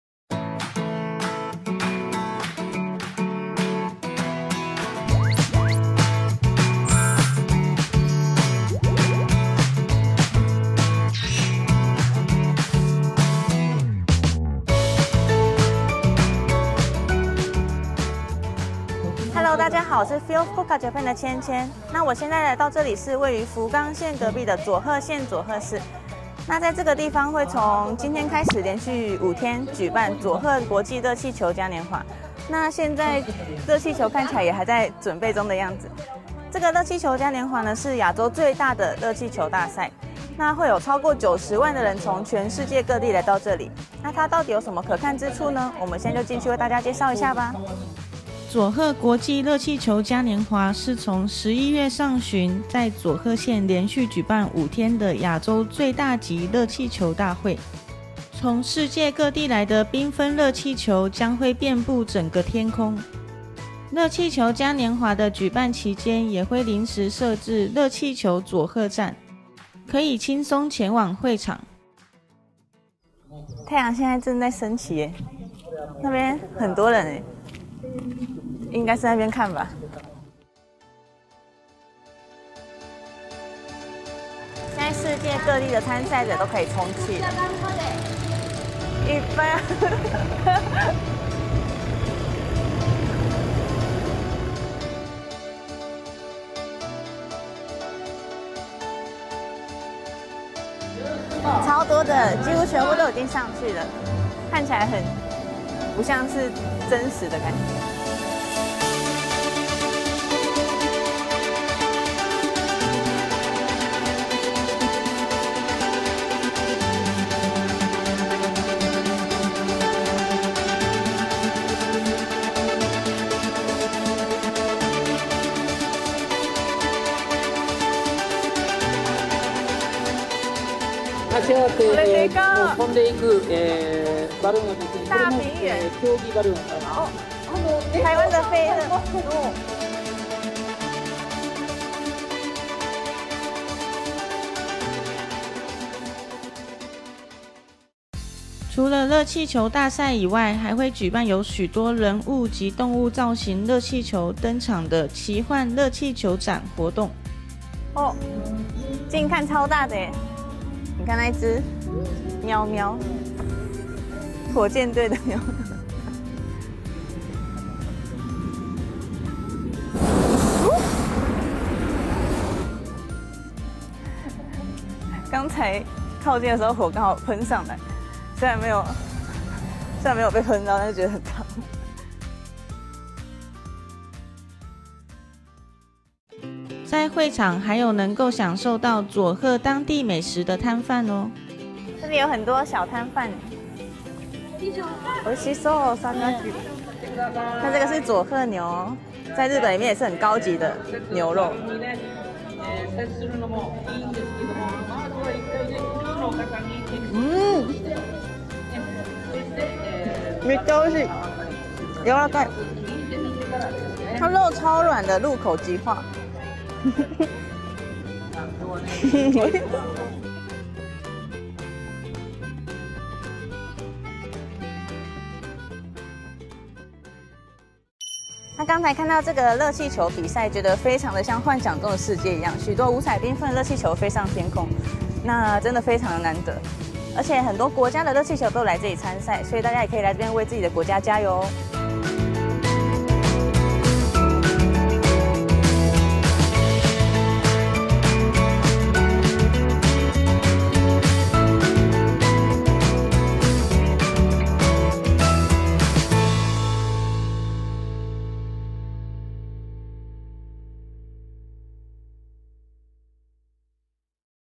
Saga International Balloon Fiesta
Asia’s largest-scale balloon festival: The Saga Balloon Fiesta
In 2018 it will be held over a period of 5 days from October 31 through November 4.
Our Chien Chien just can’t take her eyes off the sky full of colorful balloons.
If you are in Fukuoka during the festival, be sure to add it to your plans!